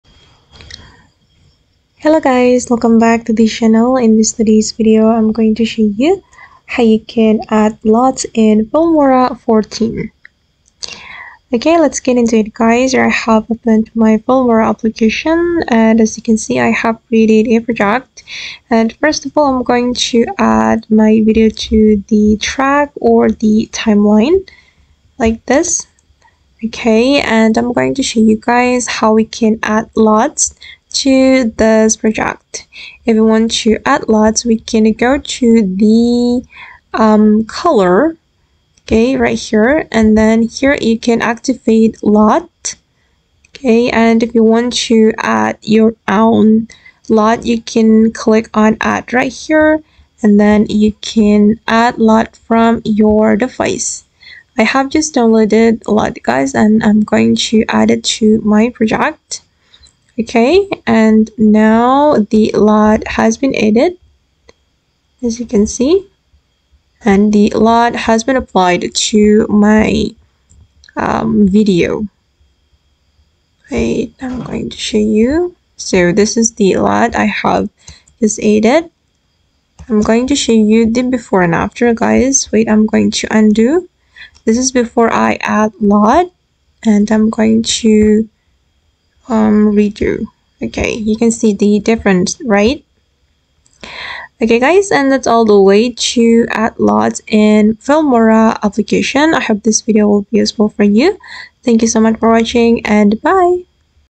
Filmora 14 Tutorial: How to Add LUTs to Your Video in Filmora 14
Want to enhance the look of your videos with professional color grading? In this Filmora 14 tutorial, we’ll walk you through how to add LUTs (Look-Up Tables) to your video for a cinematic, polished look!